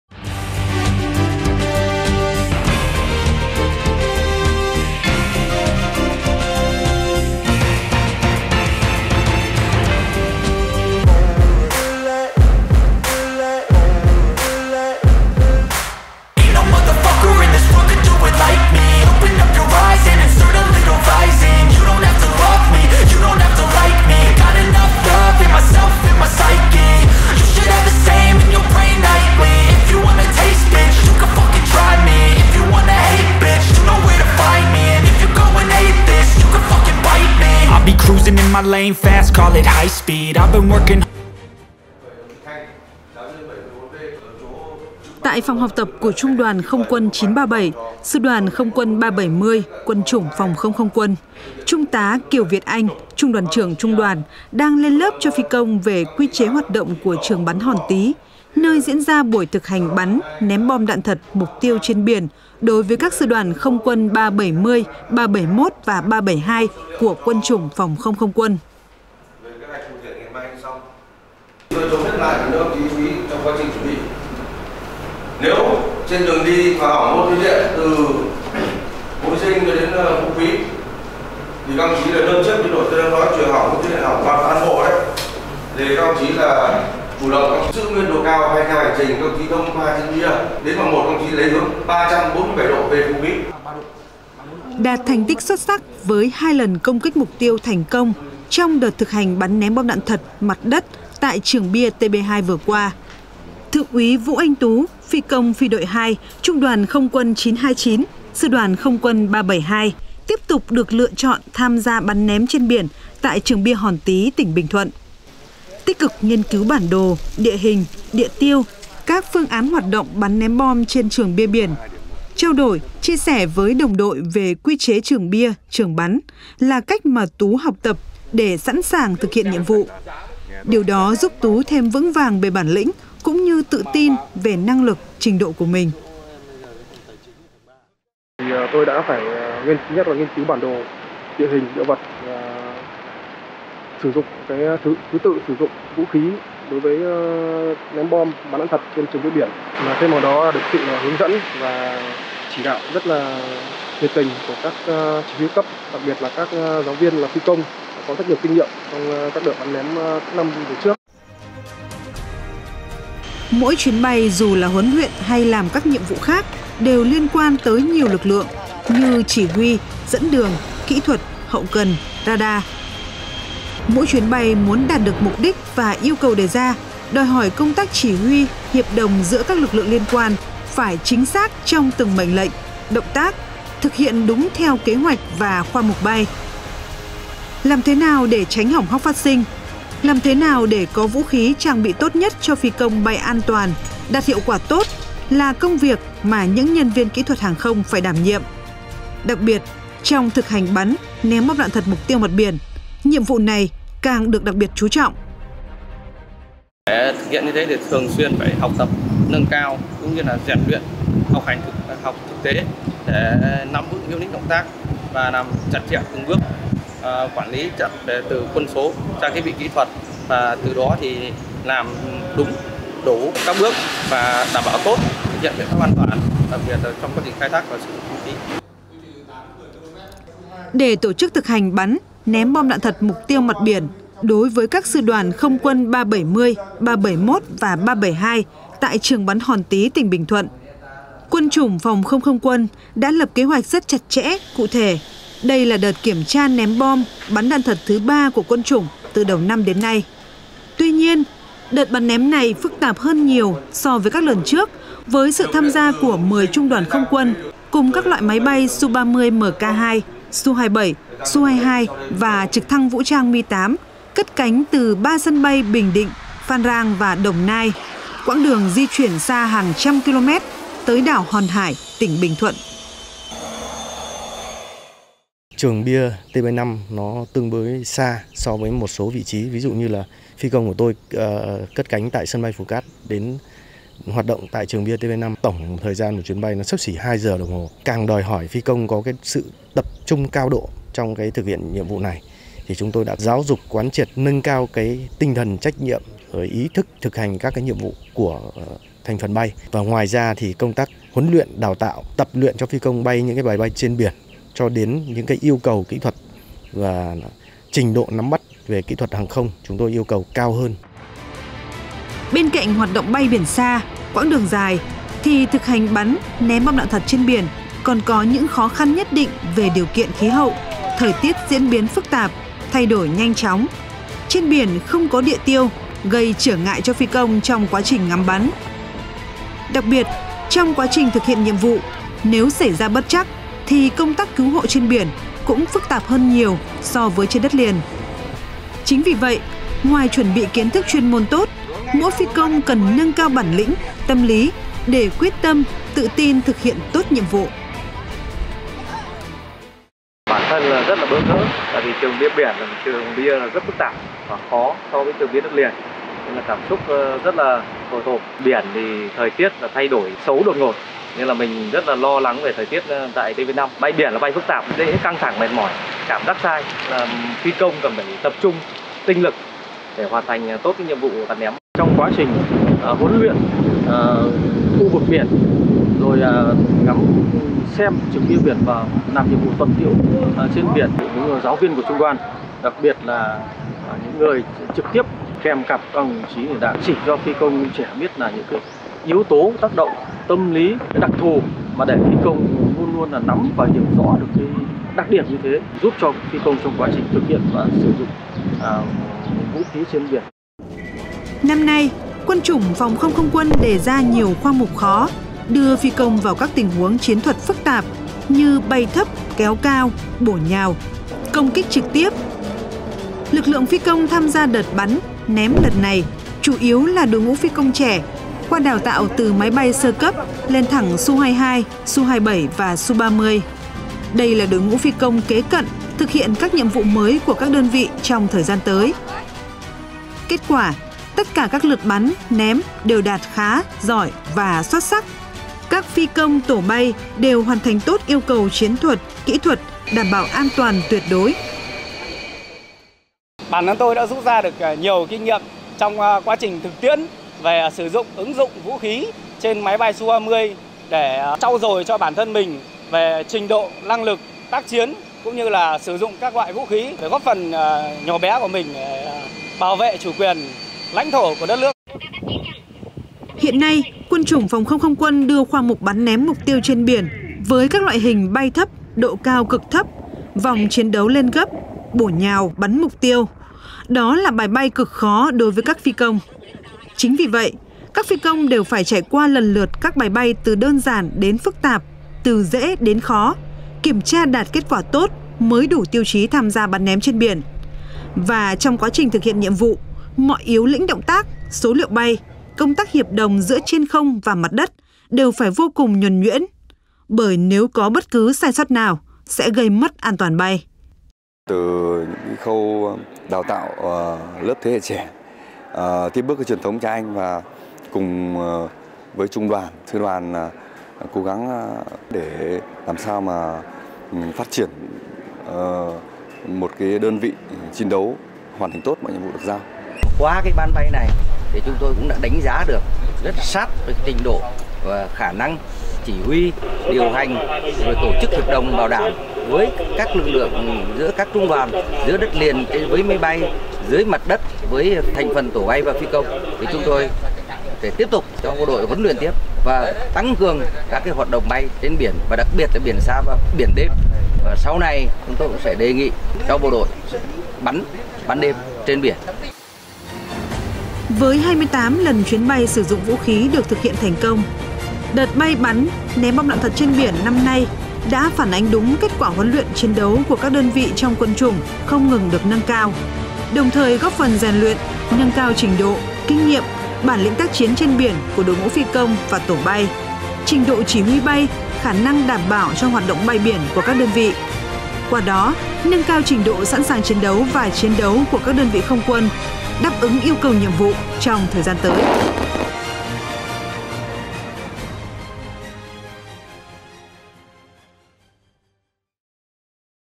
Toàn cảnh buổi huấn luyện bắn ném bom trên biển của dàn chiến đấu cơ SU-30MK2, Su-27 không quân VN
Mời quý vị và các đồng chí cùng đến với những hoạt động thực hành ném bom đạn thật trên biển của Quân chủng PK-KQ trong Phóng sự: "Luyện giỏi để sẵn sàng chiến đấu".